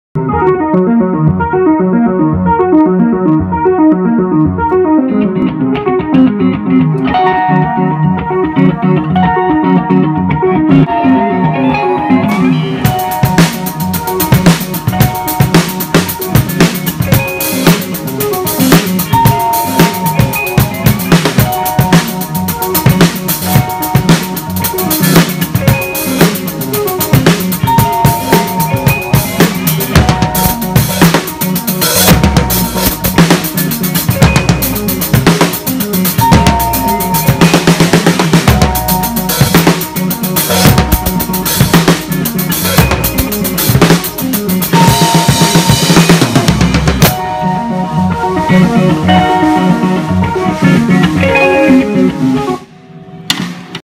Multi-Instrumental loop (Fernyloops)
created a loop using a Juno 60(80s) and fender jazzmaster(90s) going into a strymon timeline and from the timeline into a morgan ac 20, then to be followed by a fun drum beat on a vintage yamaha kit (70s).